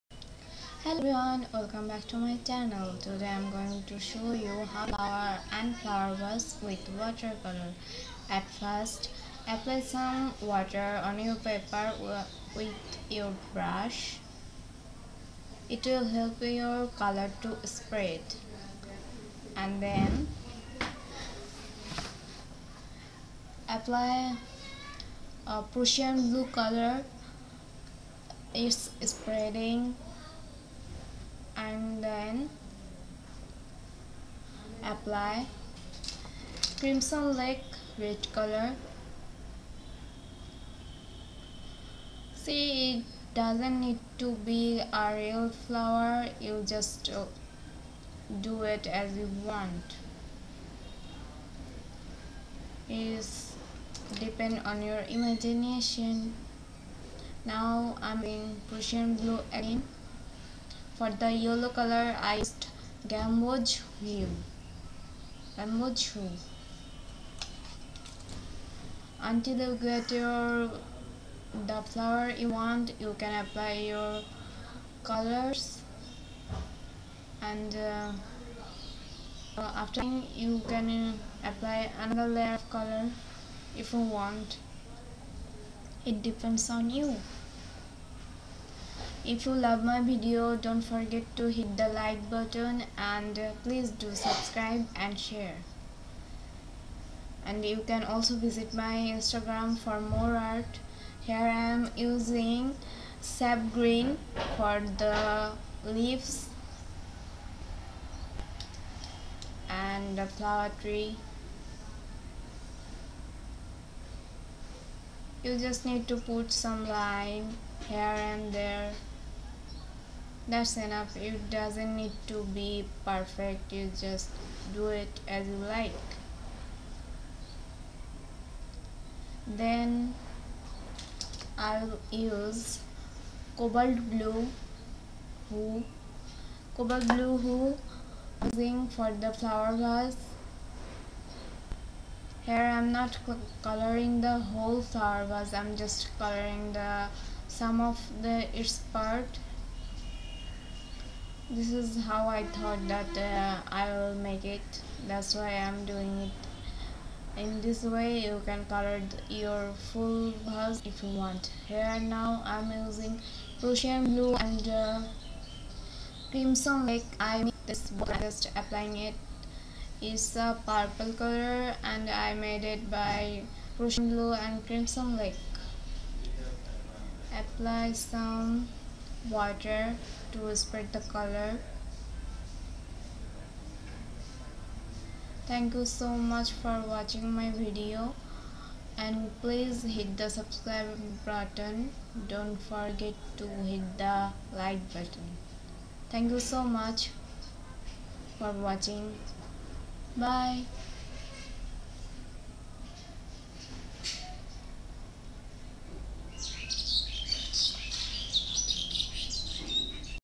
flower vase watercolor painting
in this video i showed how to draw flower and flower vase with water color.